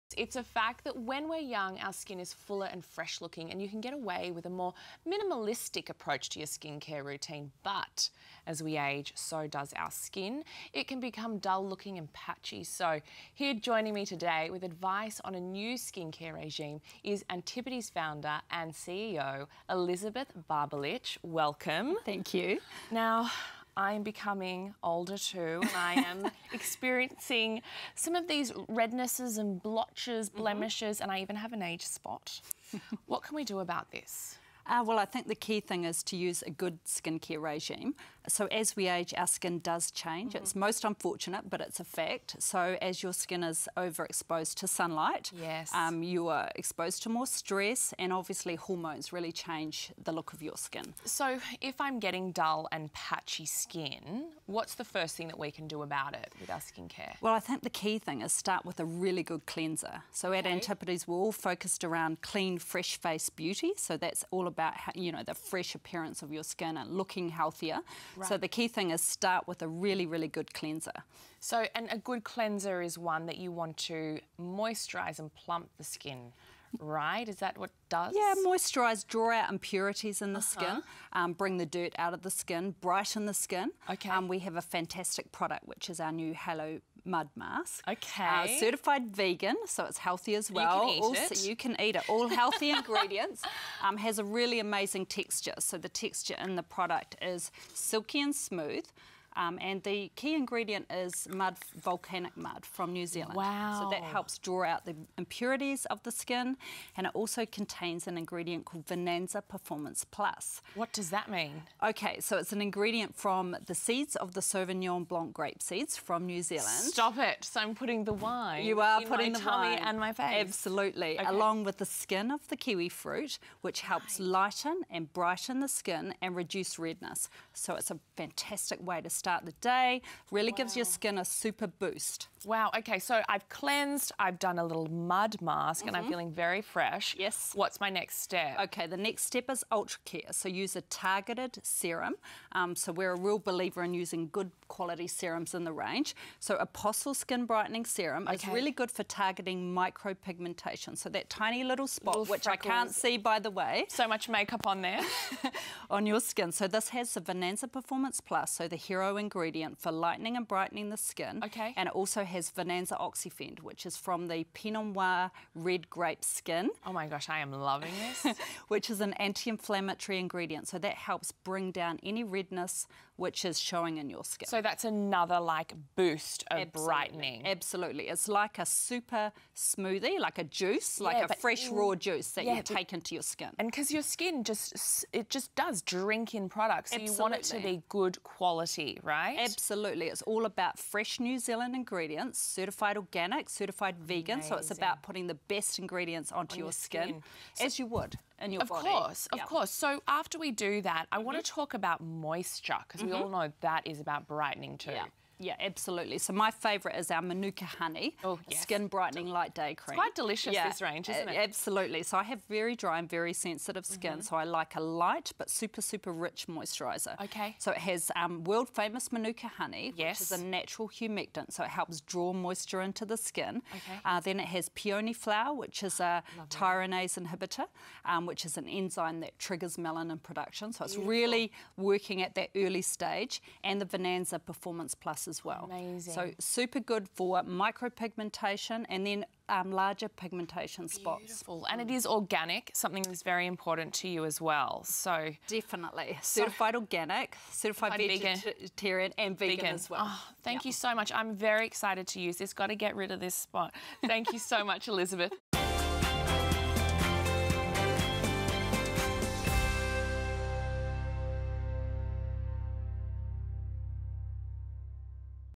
How to banish dull uneven skin
Zoe Marshall chats with CEO and Founder of Antipodes skin care range, Elizabeth Barbalich about how skin-brightening antioxidants can help revitalise the skin.

Watch LIVE each Sunday at 12 noon on Channel 7 or catch up on the episode